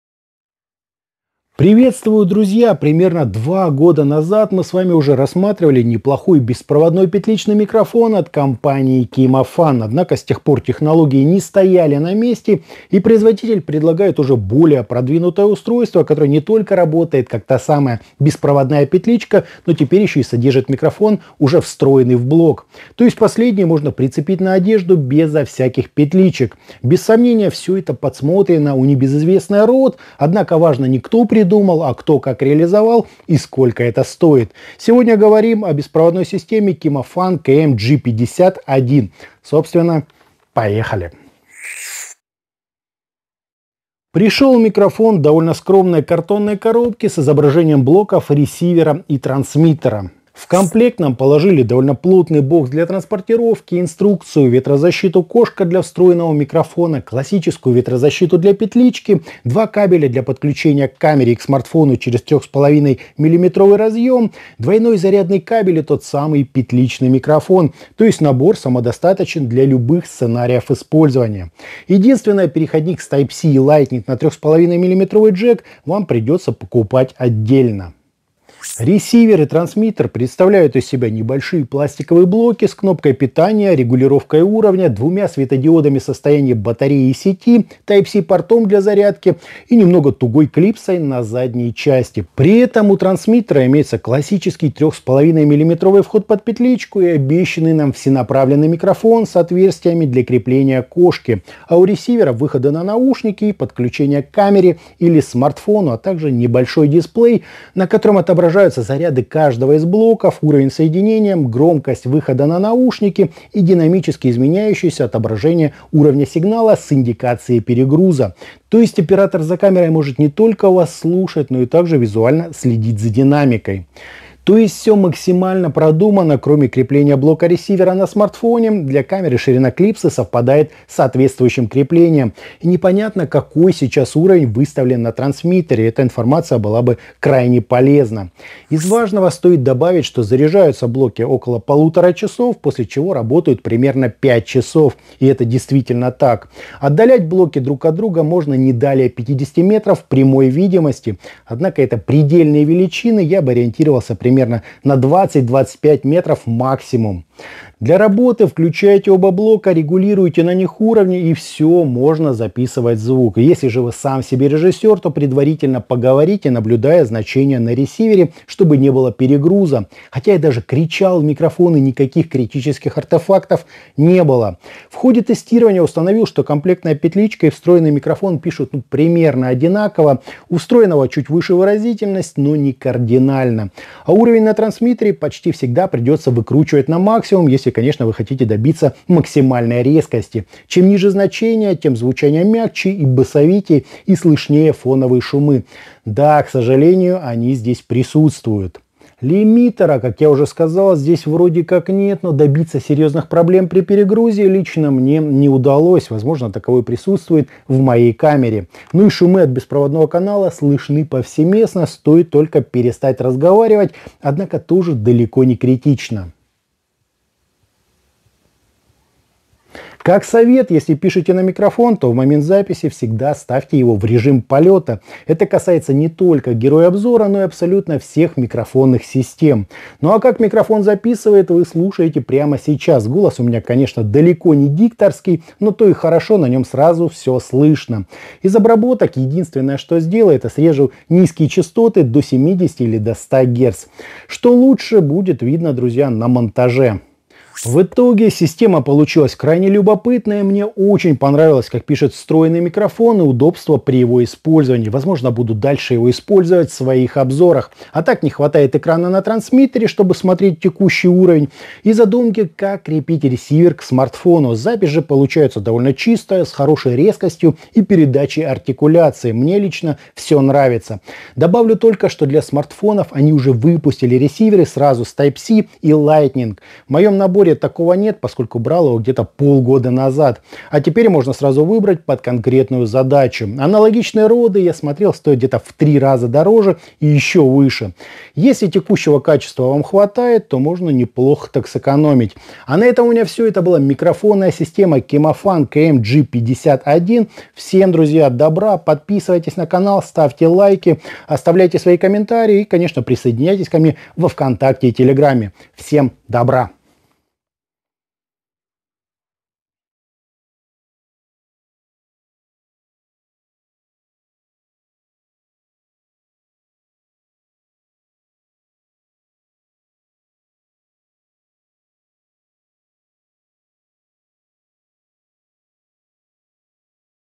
Обзор Kimafun KM-G50-1: недорогой беспроводной микрофон
✔ Звук записан на смартфон Galaxy S21 через приложение камера.

00:00 - Вступление
00:50 - Комплектация Kimafun KM-G50-1
01:18 - Внешний вид и эргономика
02:18 - Kimafun KM-G50-1 в работе
04:07 - Выводы

► Kimafun G50 - это беспроводной микрофон для ПК, ноутбука или смартфона. Суммарно, система получилась крайне любопытная. Мне очень понравилось как пишет встроенный микрофон и удобство при его использовании. Запись же получается довольно чистая, с хорошей резкостью и передачей артикуляции. Добавлю только, что для смартфонов они уже выпустили ресиверы сразу с Type C и Lightning. В моем наборе такого нет, поскольку брал его где-то пол года назад. А теперь можно сразу выбрать под конкретную задачу. Если текущего качества хватает, то можно неплохо так сэкономить.